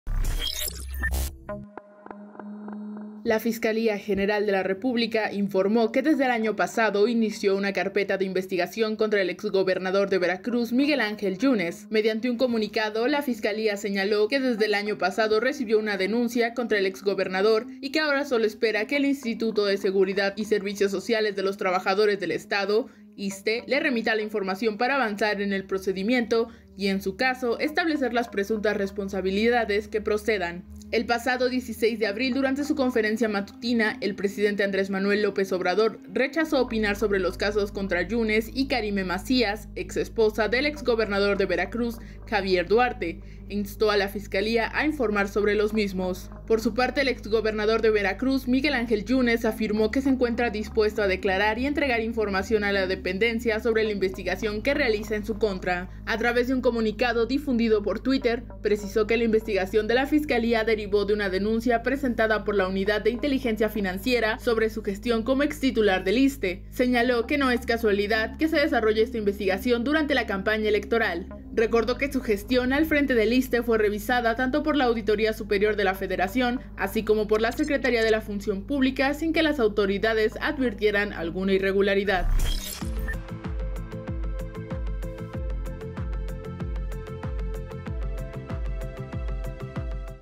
FGR inicia investigación contra Miguel Ángel Yunes por su gestión en el ISSSTE
La Fiscalía señaló que desde 2020 recibió la denuncia contra el exgobernador y espera a que el ISSSTE le remita información para avanzar en el procedimiento; la extradición de Karime Macías aún sigue pendiente